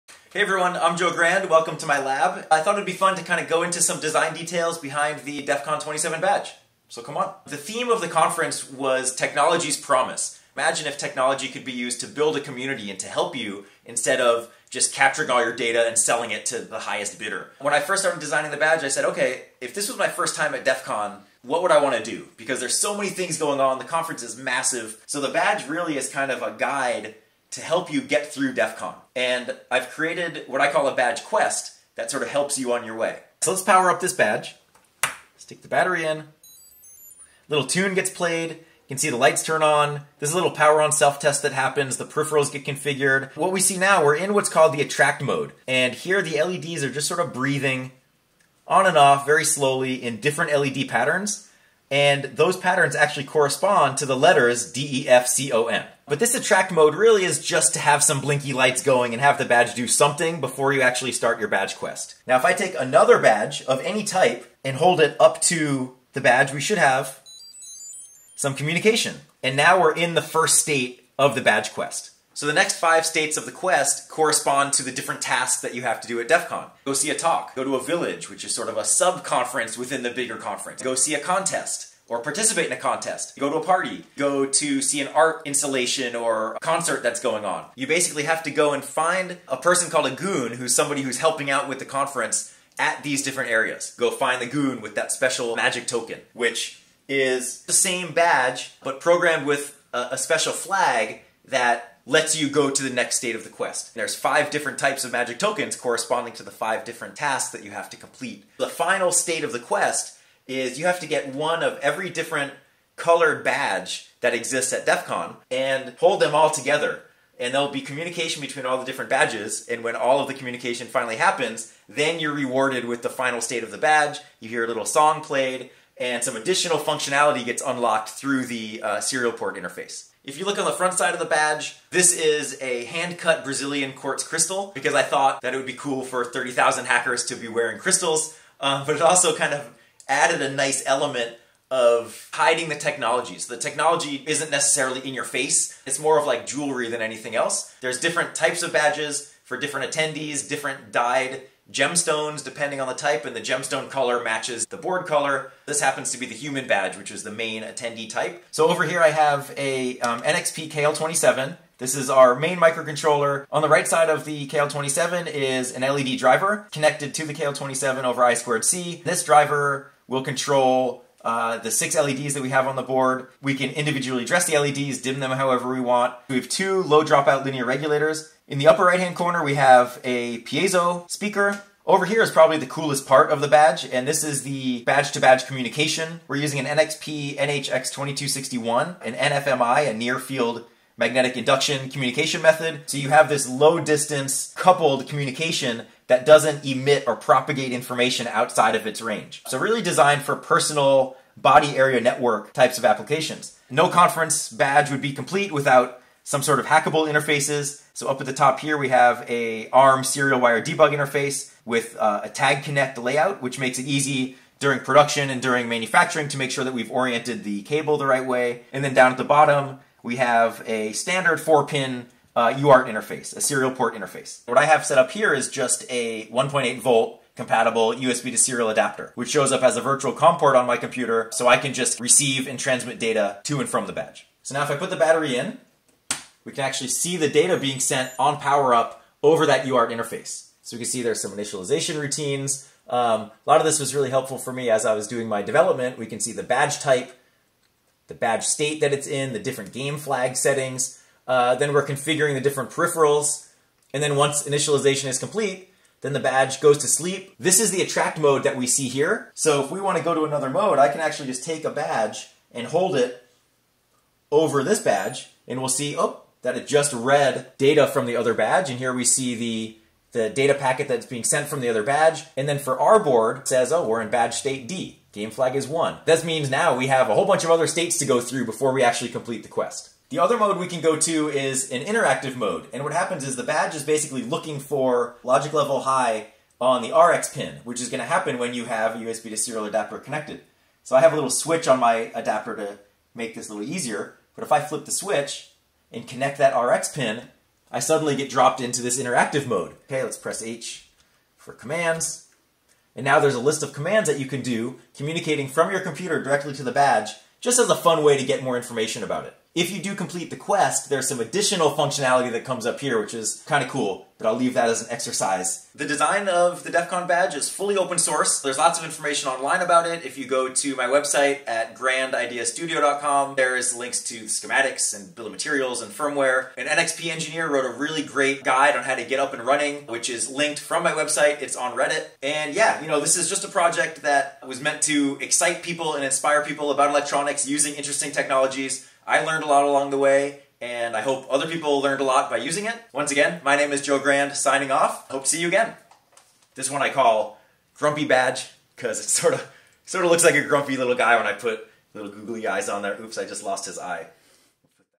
A Look at the DEFCON 27 Badge
Short story, design details, and demonstration of the DEFCON 27 Badge, the official attendee badge for @DEFCONConference, the largest and oldest continuously running hacker conference in the world.

Filmed by @NXPDeveloperZone during a visit to my lab after the event in August 2019.